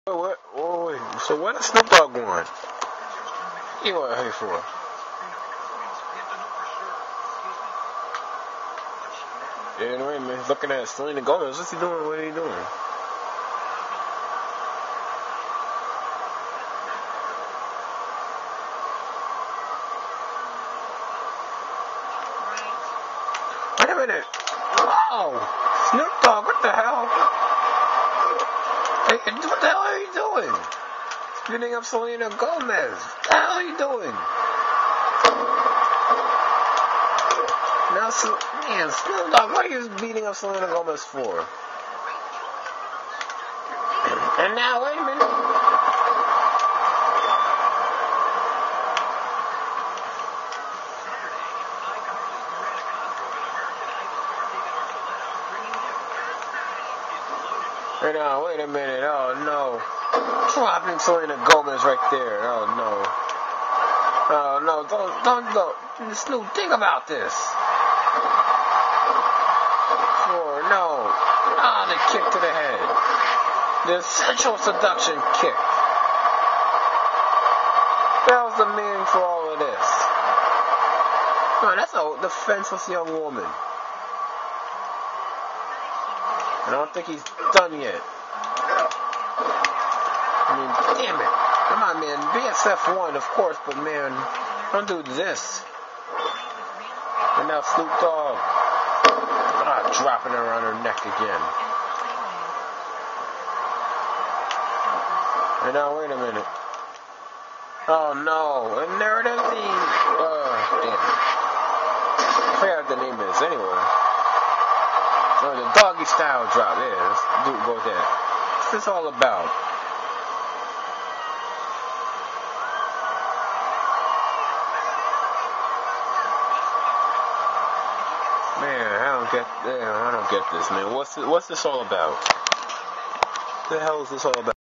LASF Episode 2 part 6c
LASF' first episode Match Card: No DQ Tag Team Match for the BSF Hardcore World Tag Team Championship. Mo-Fo Clan vs. ?

Elimination Chamber Qualifying Match: Falls Count Anywhere. Terry Tate vs. Sean Spruil.

BSF vs. LASF. 15 Mintue Ironman. BSF's Dane Cook vs. LASF's 50 Cent.

First Blood. Randy Orton v.s Undertaker.

Main Event: BSF vs. LASF. Six-Man Mixed No DQ Tag. BSF's ? & ? & ? vs. LASF's Hustlin Boyz & Selena Gomez.